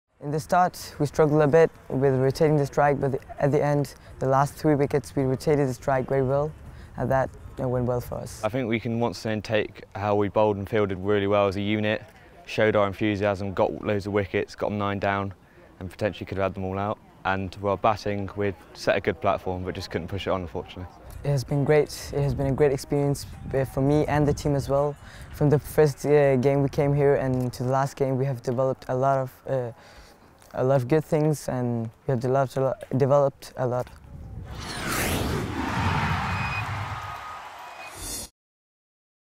U19CWCQ Europe Div 2: Guernsey v Denmark - post-match interviews
Reaction from Guernsey and Denmark after their third-place play-off at U19CWCQ Europe Division 2, where the Danish side sealed promotion with a 98 run win.